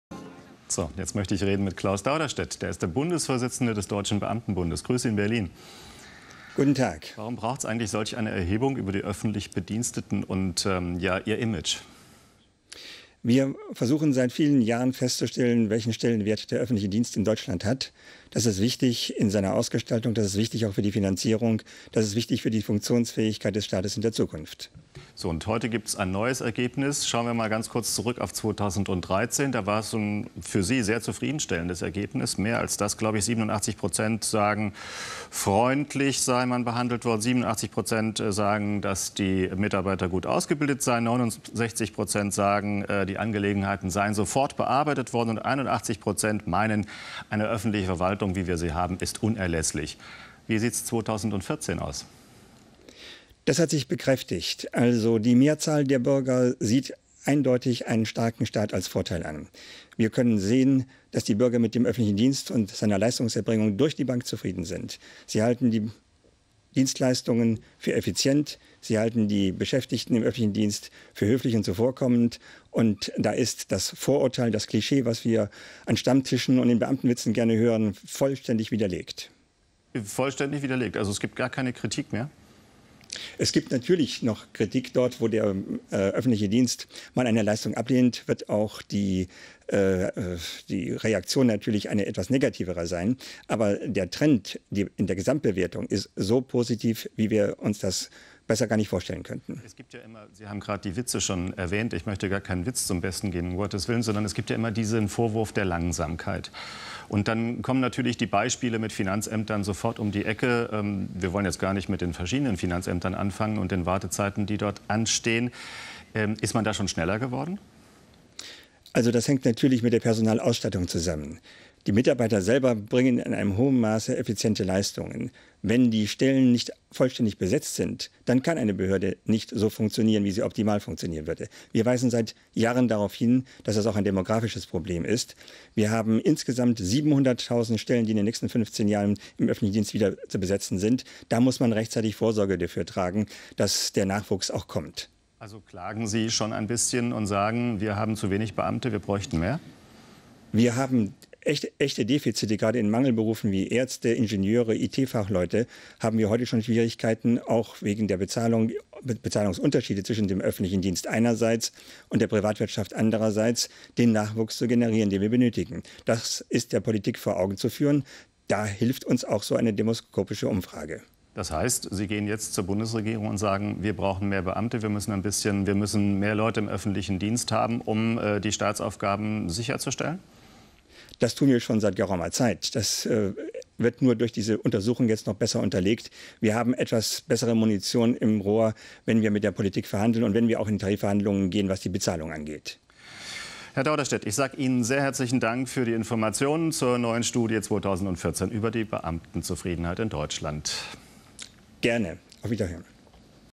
dbb-Umfrage: Klaus Dauderstädt zur Zufriedenheit mit Öffentlichen Dienst am 26.08.2014
Stephan Kulle im Gespräch mit Klaus Dauderstädt (dbb-Bundesvorsitzender) über die Umfrage zum Ansehen vom Öffentlichen Dienst in Deutschland.